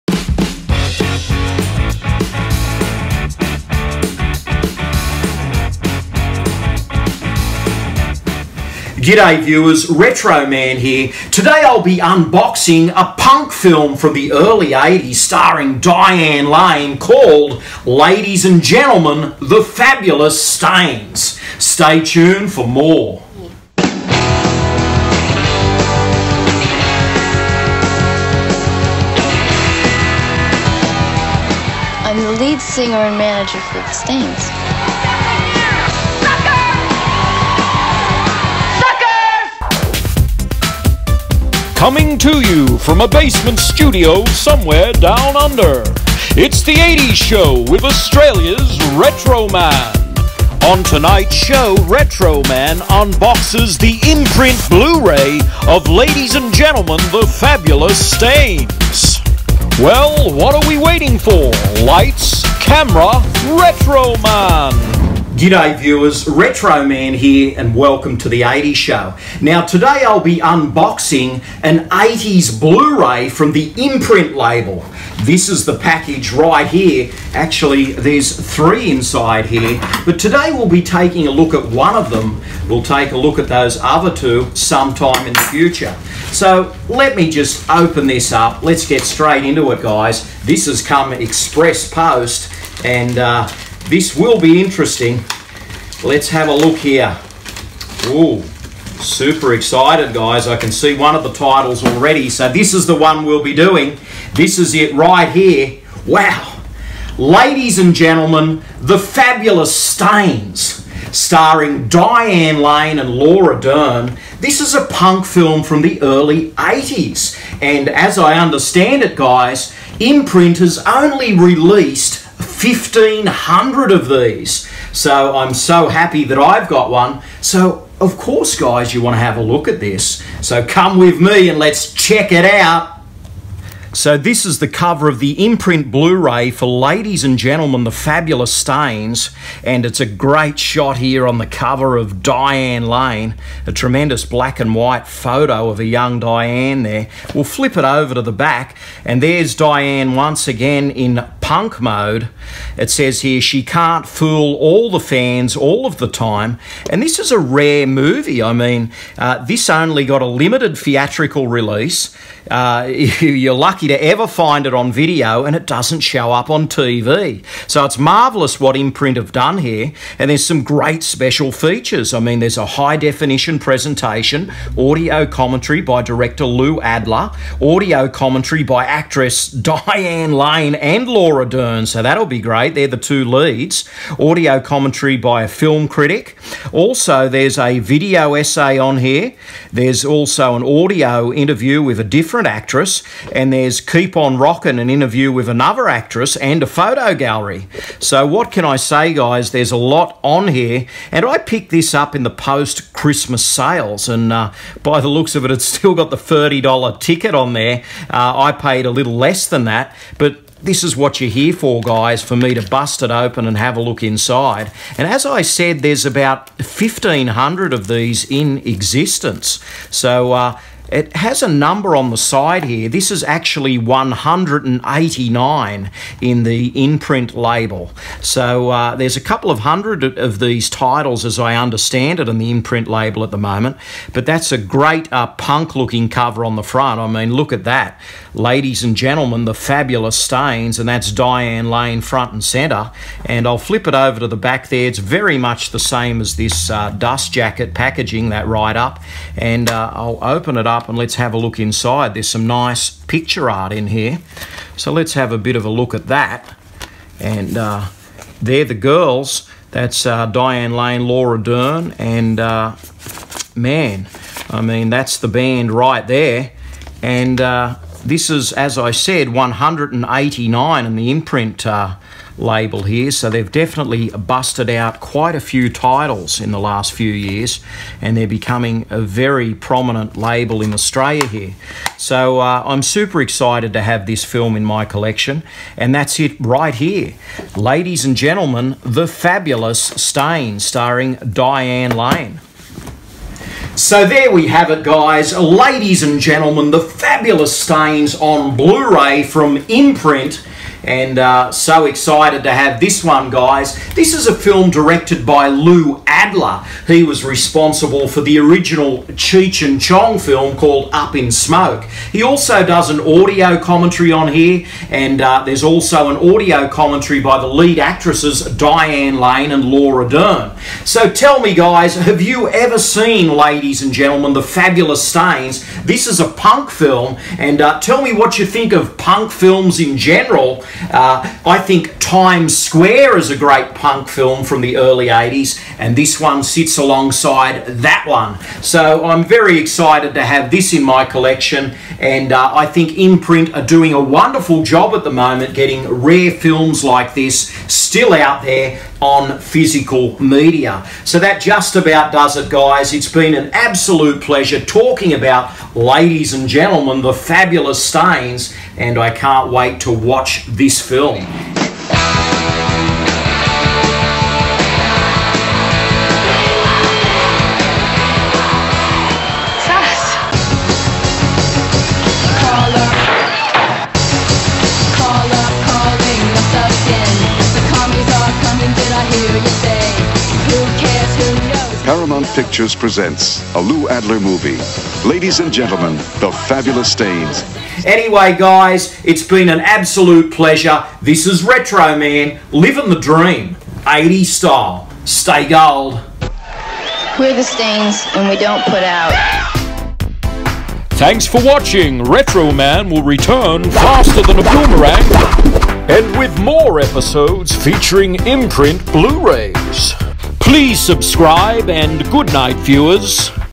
Ladies And Gentlemen The Fabulous Stains 1982 Imprint Blu-ray Unboxing Diane Lane
In this episode of The 80s Show, Retro Man unboxes the Blu-ray of the classic punk rock movie, Ladies And Gentleman The Fabulous Stains (1982)  from Imprint Films. This rare rock n roll flick directed by Lou Adler stars Diane Lane, Laura Dern and Ray Winstone. Please share your thoughts on Diane Lane and The Fabulous Stains in the comments below.

The 80s Show with Australia's Retro Man brings you a rad feast of Movies, Music, Memories and Munchies. Stay Gold!